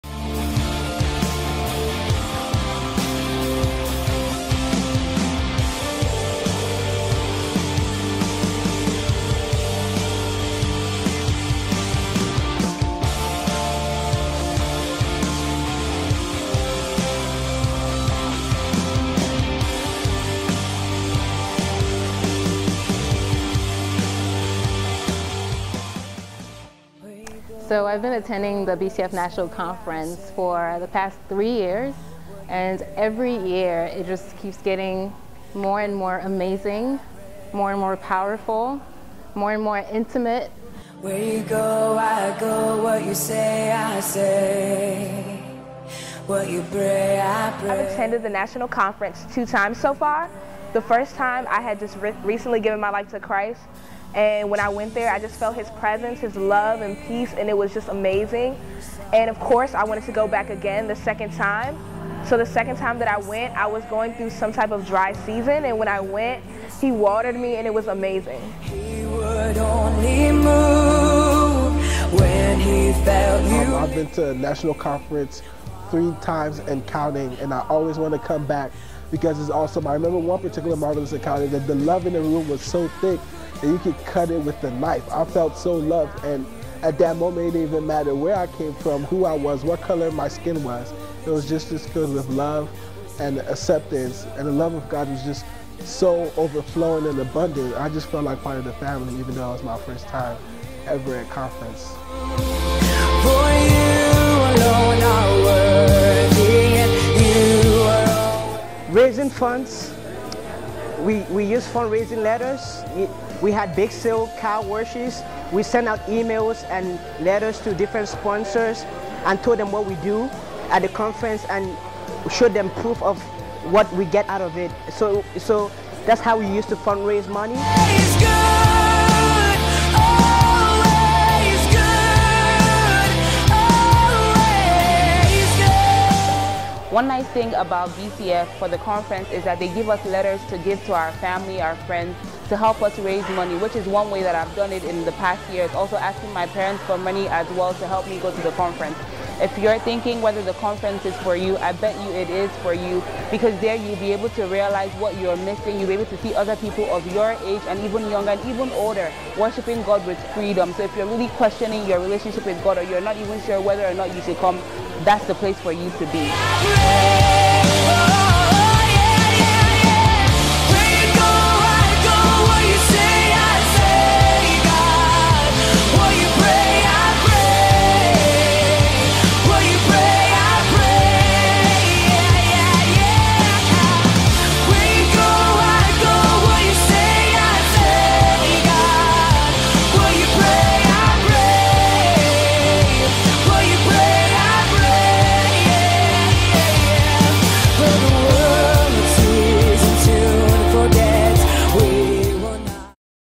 Promo 2 | BCF National Conference - 2014
WHEN GOD SAVES A MAN HE CALLS HIM INTO SERVICE. LIKE IN THE DAYS OF MOSES AND ISAIAH GOD IS STILL SAVING STUDENTS. HE IS STILL EMPOWERING THEM TO RESPOND TO THE DEPRAVITIES THAT DOMINATE COLLEGE LIFE TODAY IN THE USA AND AROUND THE WORLD.

IN FEBRUARY 2014 STUDENTS FROM THE UNITED  STATES AND AROUND THE WORLD WILL GATHER AT RIDGECREST NORTH CAROLINA TO BE TRANSFORMED AND EMPOWERED BY THE SPIRIT OF GOD TO BE A DELIVERER OF STUDENTS HELD UNDER THE CAPTIVITY OF SIN.